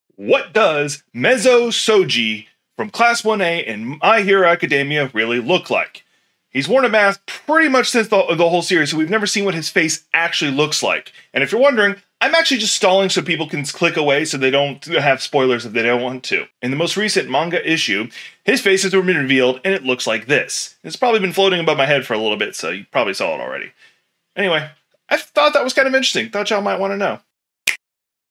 Mezo Shoji from My Hero Academia face reveal
Mezo Shoji has worn a mask throughout his tenure in Class 1A. It has been a question what his face really looked. Considering his powers are so unusual it could a lot of possibilities.

Well in the MHA manga he finally has had a face reveal.

I now have a tip jar, feel free to not donate.